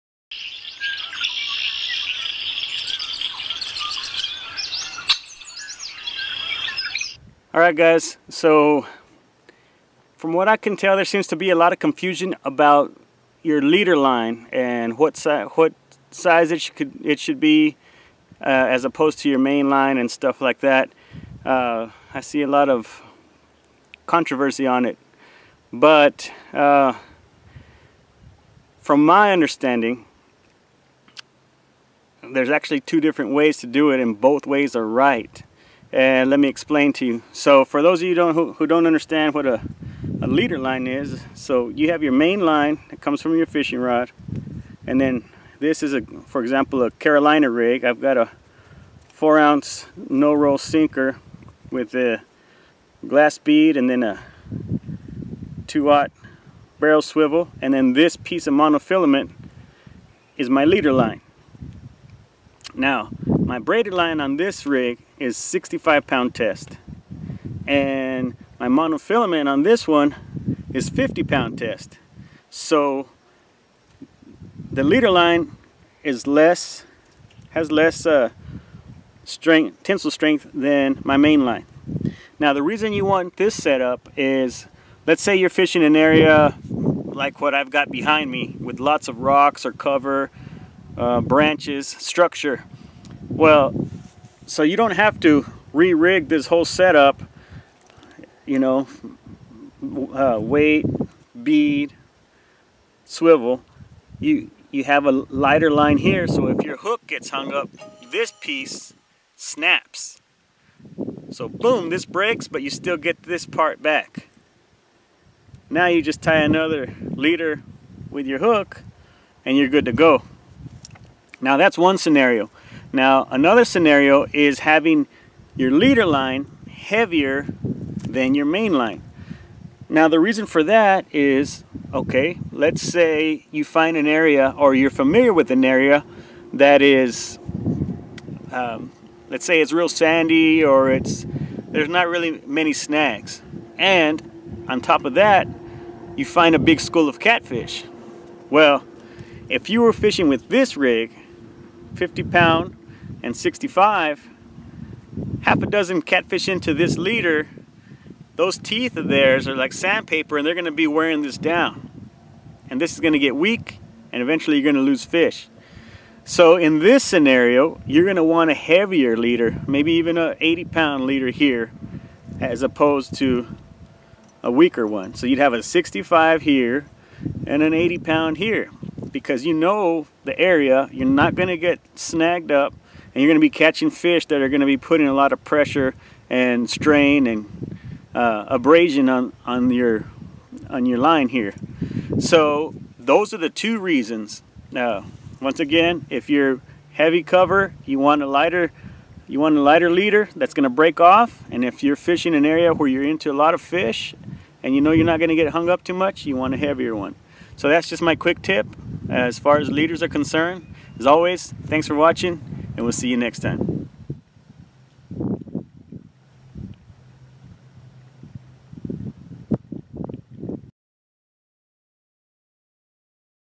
The Purpose Of Leader Lines
Using a leader line when fishing for catfish can be very beneficial. A lighter leader is ideal for fishing heavy cover to help prevent losing your entire rig, while a heavier leader line can stand more abrasion if you are catching lots of fish in a short period of time.